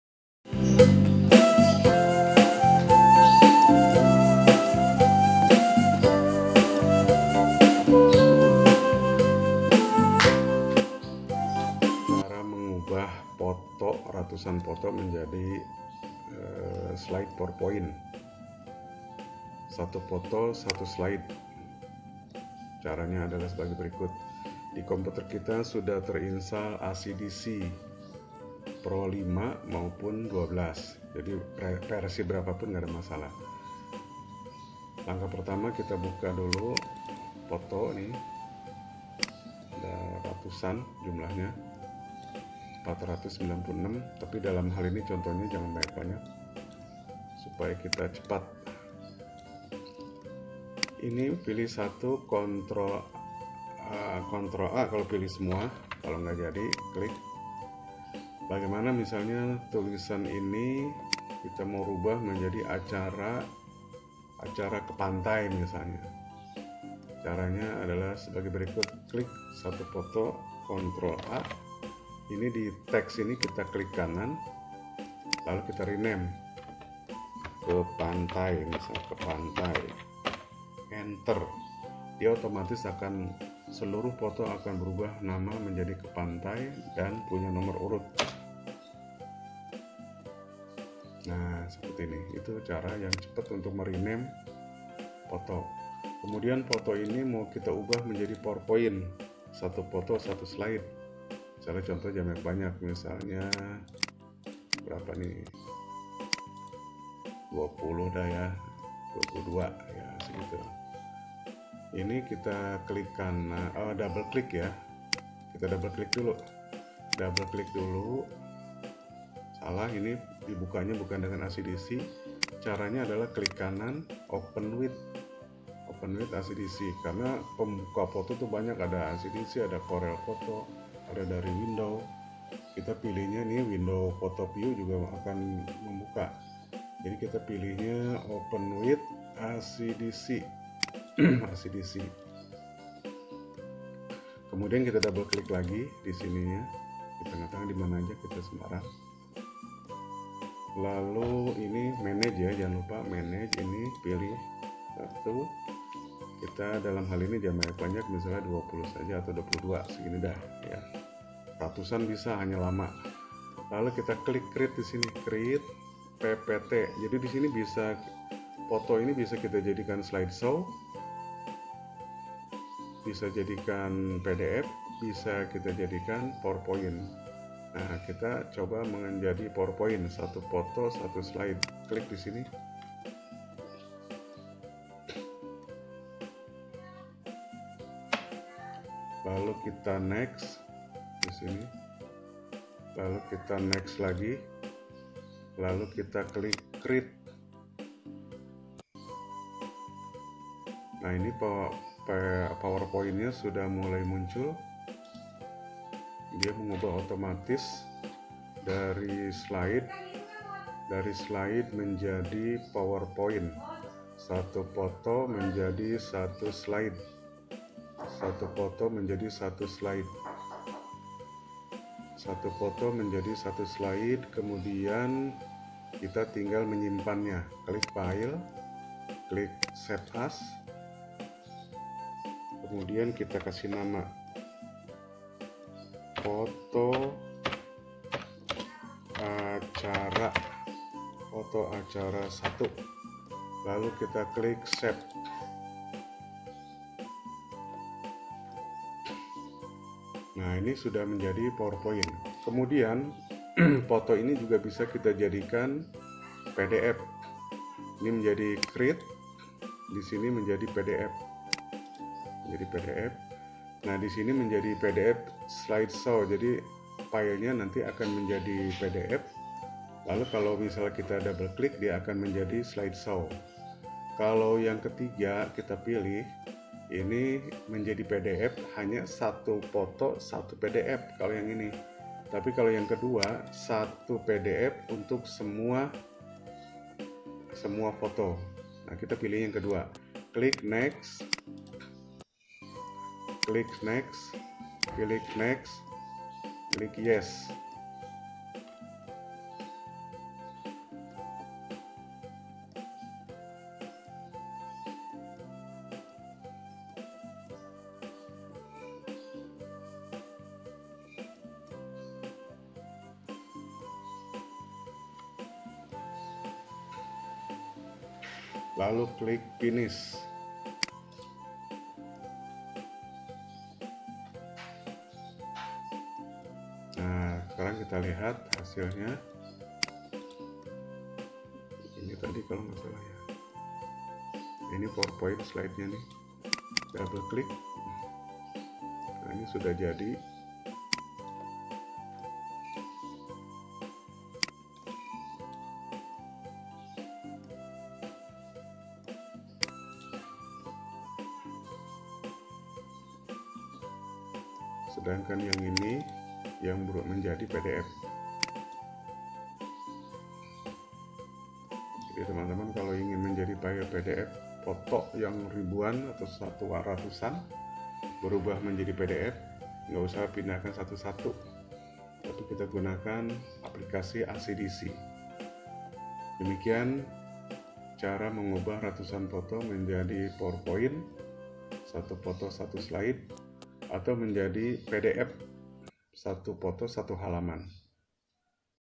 RATUSAN GAMBAR KE SLIDE PPT DENGAN ACD SEE
DENGAN ACDSEE  KITA BISA MENGCREATE RATUSAN FOTO MENJADI POWERPOINT, 1 foto 1 slide dalam waktu yang singkat, juga dari ratusan foto ke pdf dan slide show